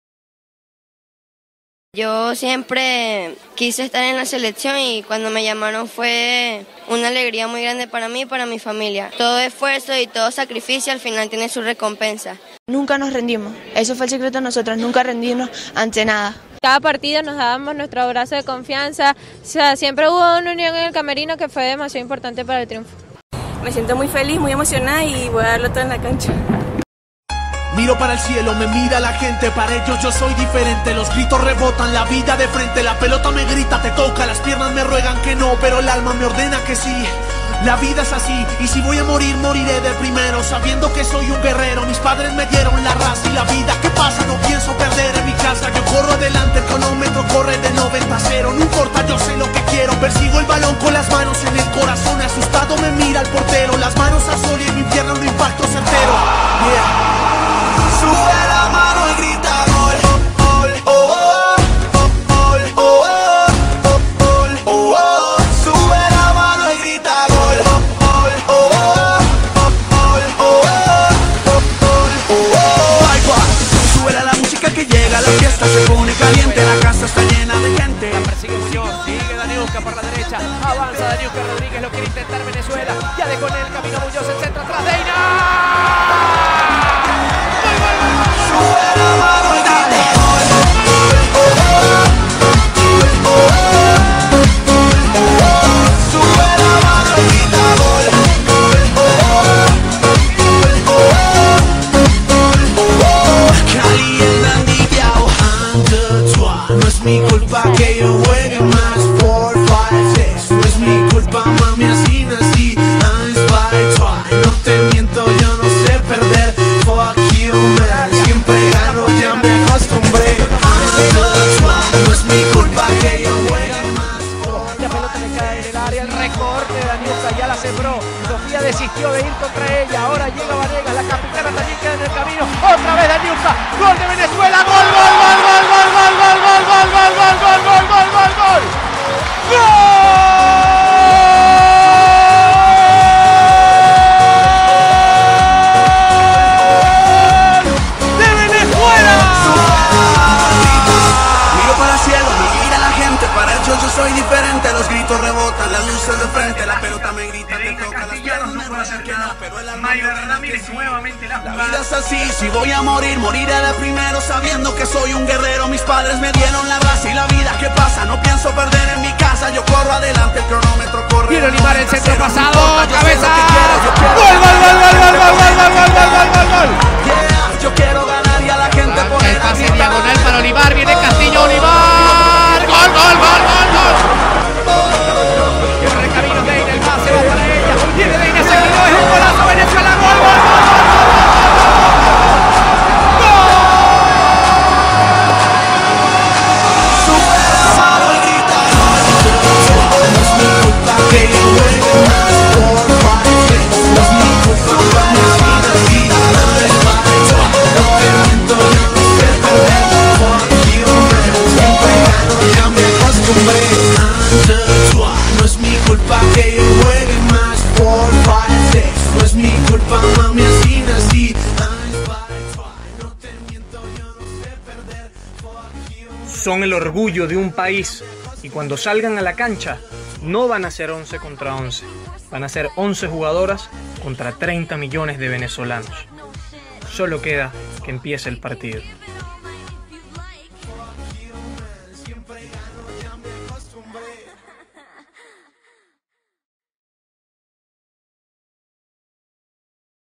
Video motivacional - Vinotinto femenina sub-17
Este fue el video que se le presentó a las jugadoras de la selección nacional previo al choque ante Paraguay, en un duelo correspondiente a la segunda fecha del cuadrangular final.